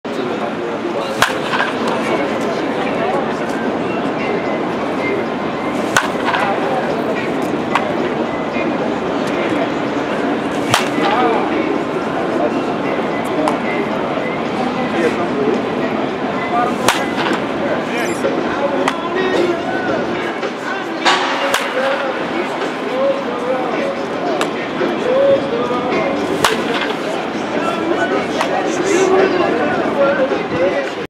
Javier Baez — Futures Game BP
Cubs prospect Javier Baez takes batting practice at the 2014 Futures Game.